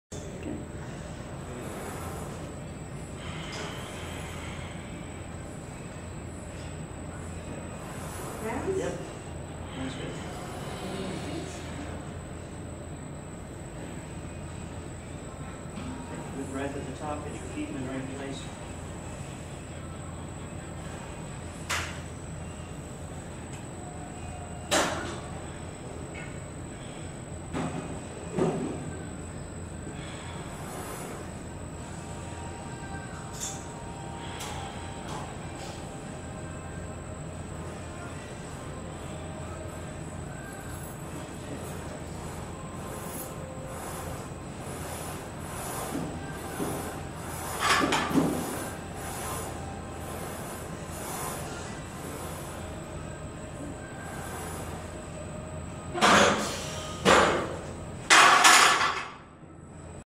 150621 Elly 115 squat
Elly van Laar 115 lb squat at BW 108 lb